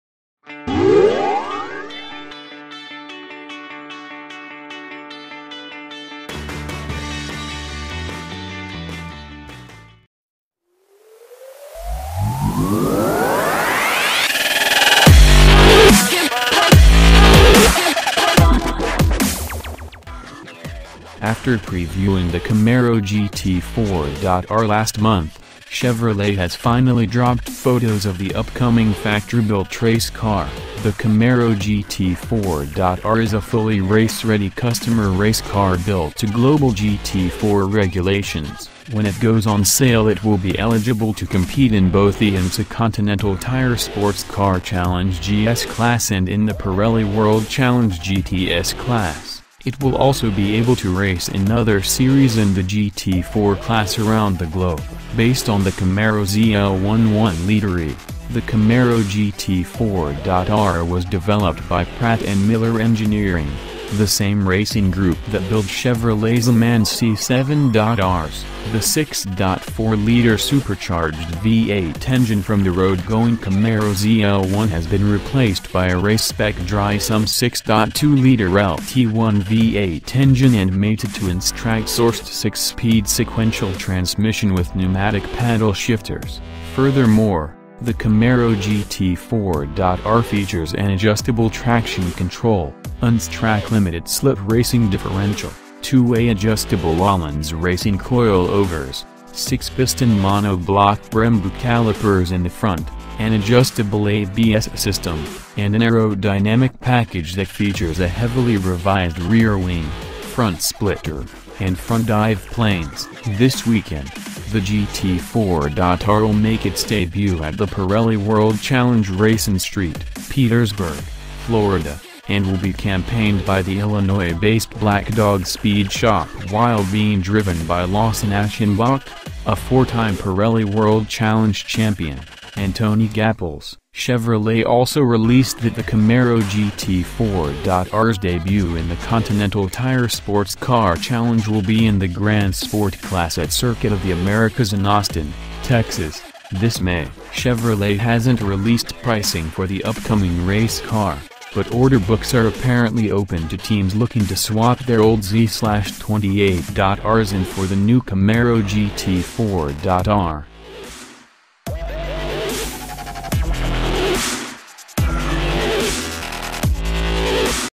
Look This!!! Chevrolet Camaro GT4 R Is Ready To Race
After previewing the Camaro GT4.R last month, Chevrolet has finally dropped photos of the upcoming factory-built race car. The Camaro GT4.R is a fully race-ready customer race car built to global GT4 regulations. When it goes on sale it will be eligible to compete in both the IMSA Continental Tire SportsCar Challenge GS Class and in the Pirelli World Challenge GTS class. It will also be able to race in other series in the GT4 class around the globe.